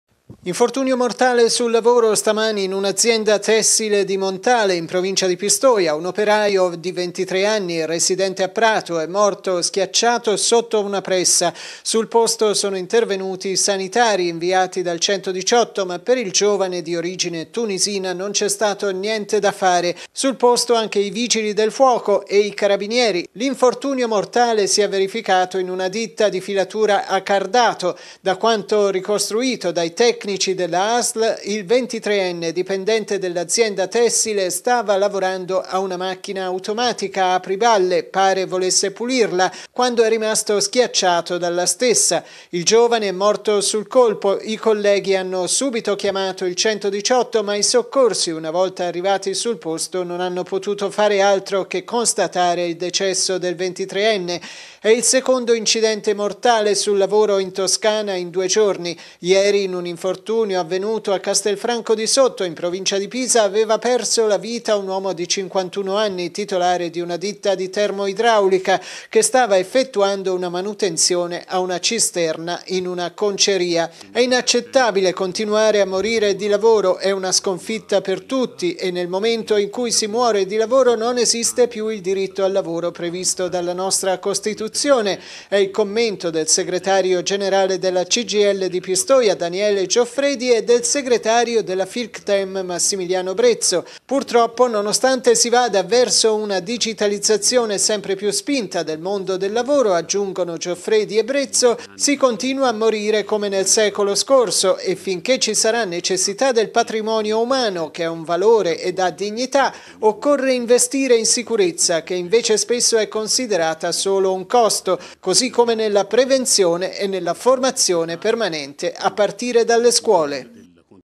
2021-02-02 MONTALE (PT) - GIOVANE OPERAIO DI 23 ANNI MUORE SCHIACCIATO DA PRESSA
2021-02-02 MONTALE (PT) - GIOVANE OPERAIO DI 23 ANNI MUORE SCHIACCIATO DA PRESSA